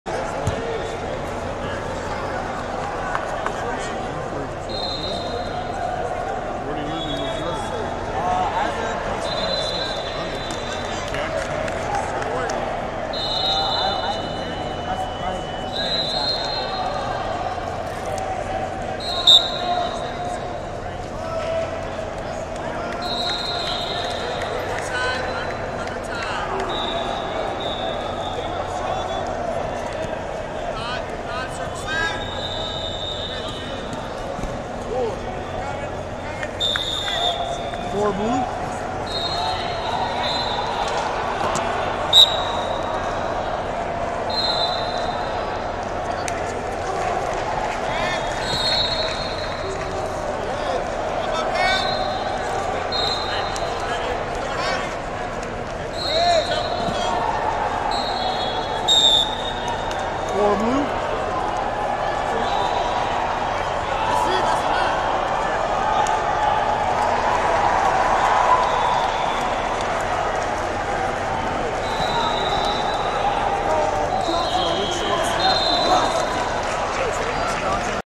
132 Lbs 4th Wrestleback (16 Team) - Javarien Jeffries, Missouri Vs Sammy Spaulding, New Jersey A98
Flosports-partner-access/2023 16U National Duals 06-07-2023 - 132 Lbs 4th Wrestleback (16 Team) - Javarien Jeffries, Missouri Vs Sammy Spaulding, New Jersey A989